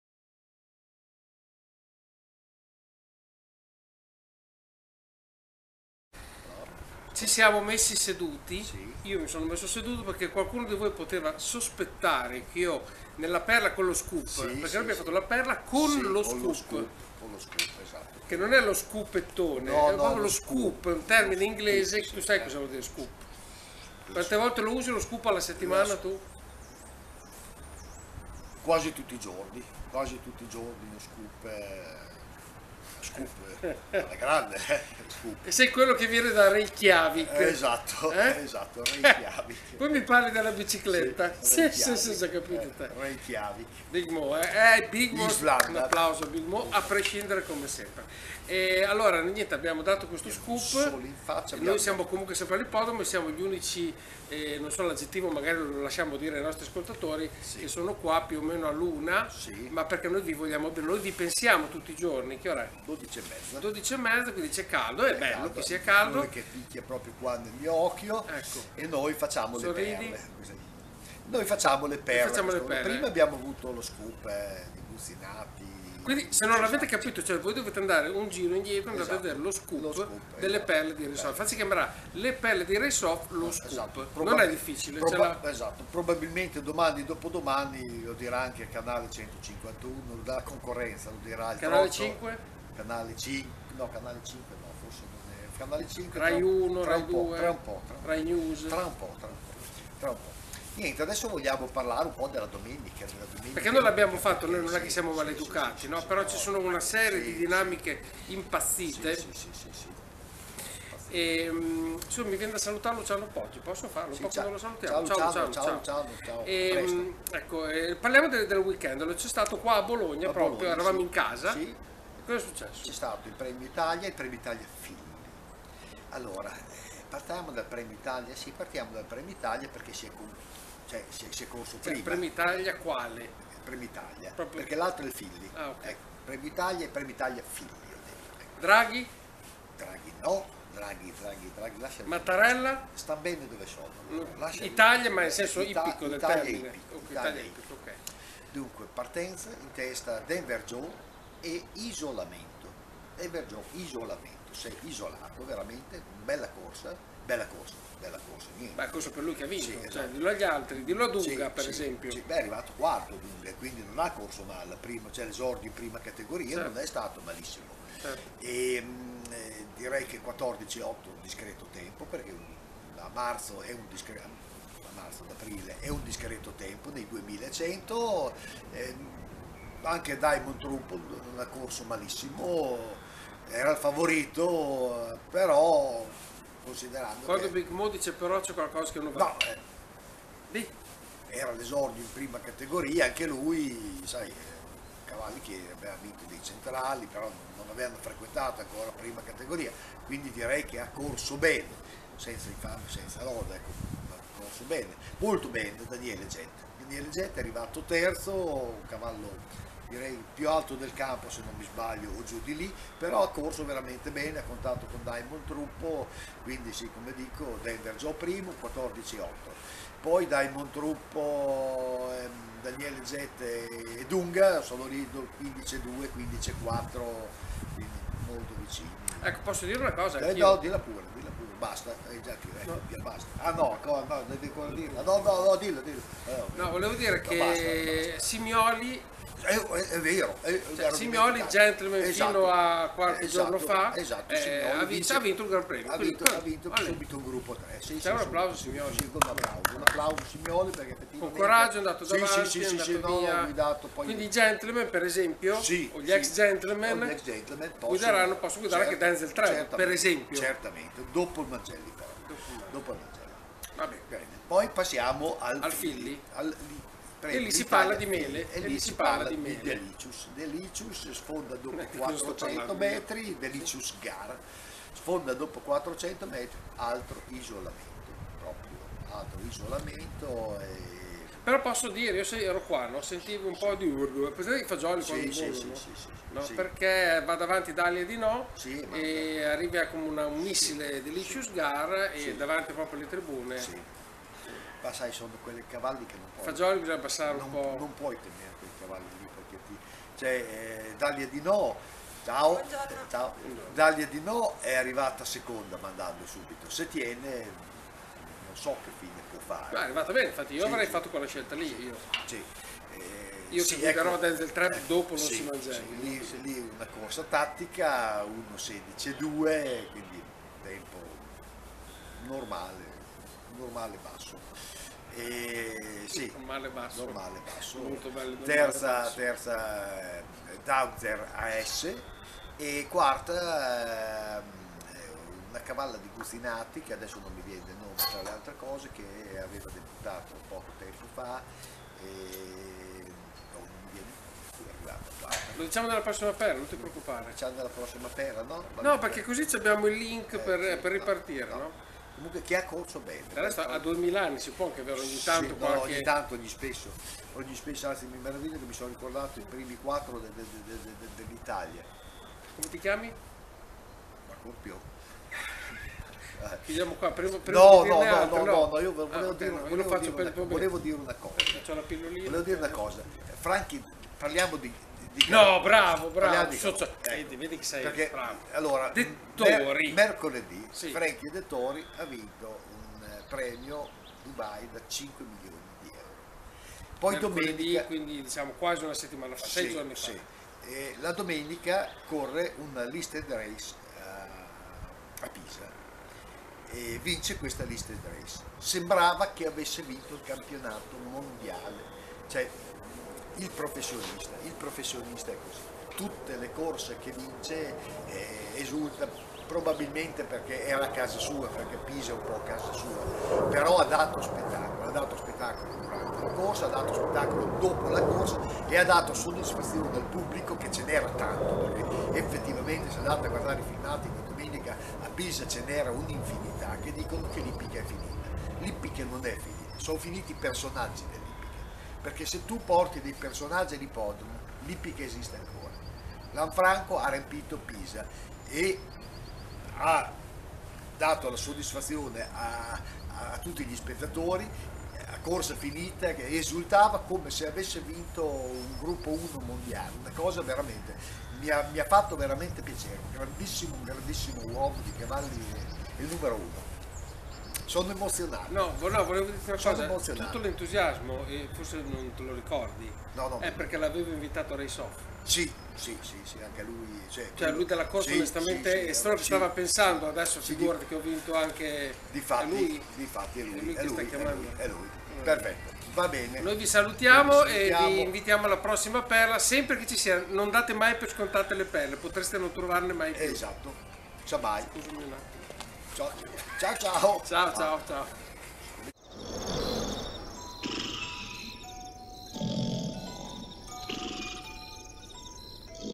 Le PERLE di RACE OFF (Ep. 7.3): L'Italia
Dallo SCOOP al Gran Premio Italia di Bologna il passo è breve soprattutto se registri le Perle sempre nel parterre dell'Arcoveggio ...